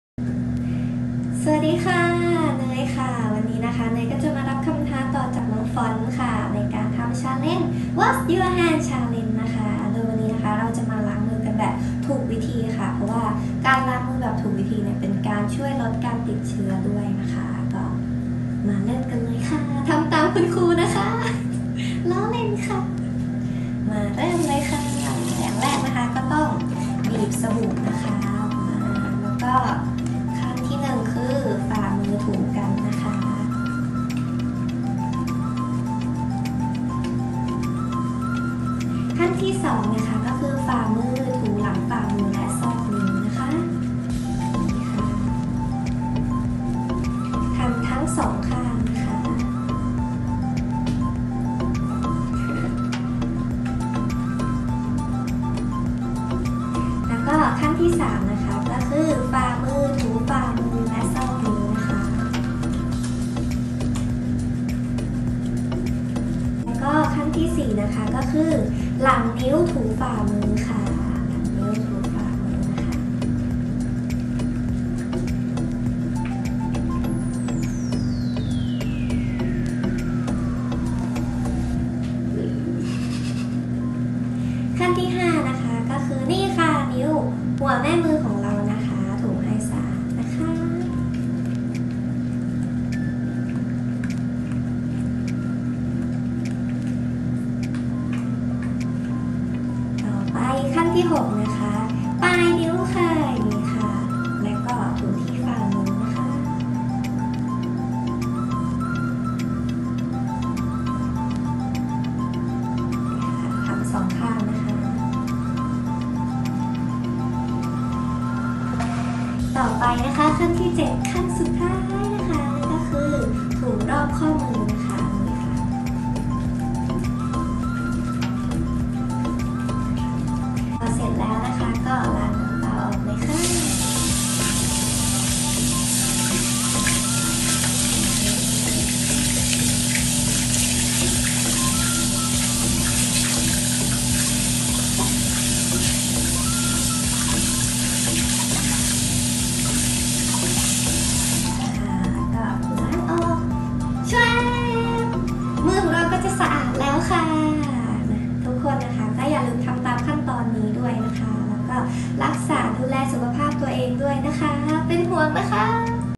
washyourhandchallenge by Noey BNK48
ล้างมือได้น่ารักเต็ม100ไปเลยค่ะ
washyourhandchallenge รับคำท้าจากน้อง